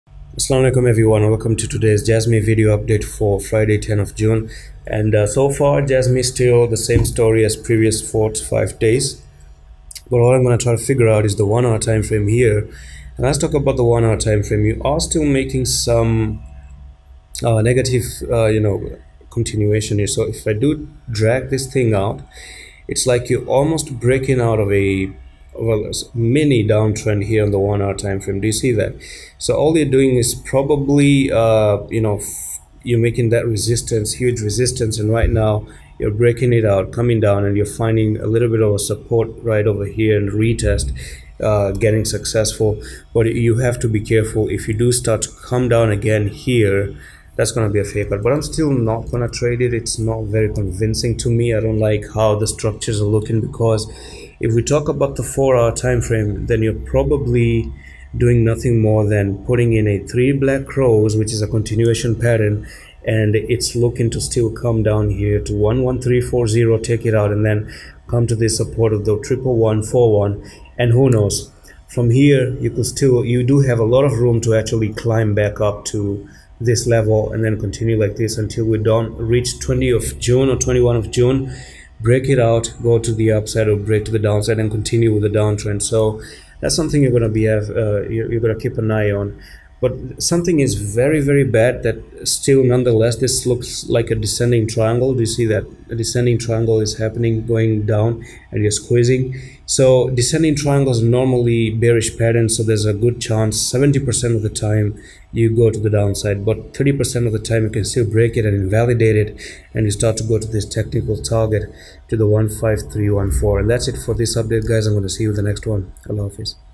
JASMY COIN UPDATE, [ BORING MOVEMENT]
Welcome back to another JASMY COIN PRICE PREDICTION update, today the story is a different one, and as candles appear we read the story let's see what it has to say today.

In this analysis you will know the next targets for coin

Here’s what I’m going to cover in this video:

JASMY COIN PRICE PREDICTION

JASMY targets

JASMY support and resistance

JASMY PREDICTION

JASMY  technical analysis

JASMY CRASH AND PUMP ETC